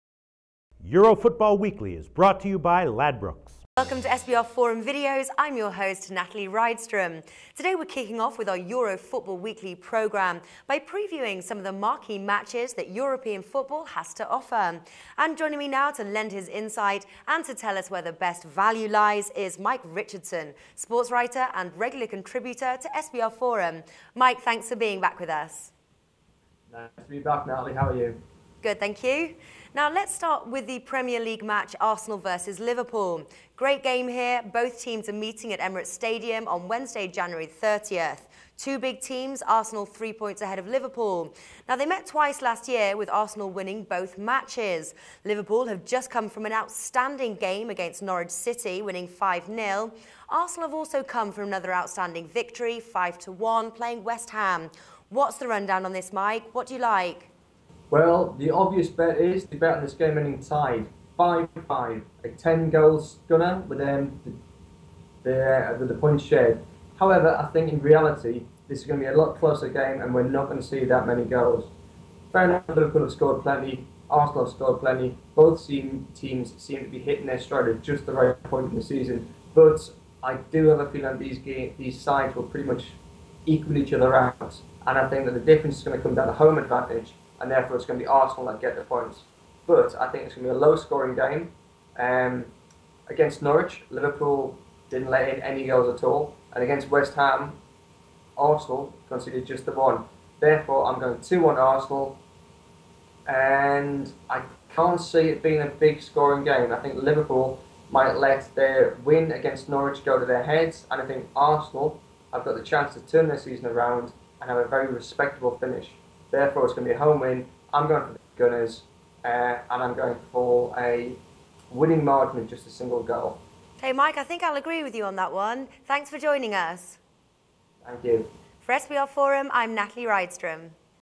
Arsenal vs Liverpool 2013: Euro Football Weekly
SBRForum's Natalie Rydström previews the Premier League matchup Arsenal vs Liverpool and discusses the betting odds with guest Mike Richardson, sportswriter and regular contributor to SBRforum.